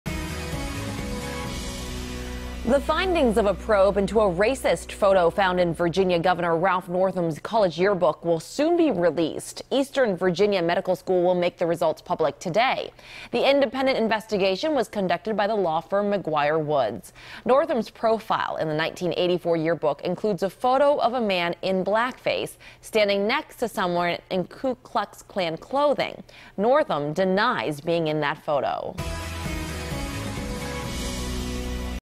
Investigating Into Virginia Gov. Ralph Northam's Racist Yearbook Photo Complete
The findings of an investigation into how a racist photo appeared on a yearbook page for Virginia Gov. Ralph Northam are set to be released.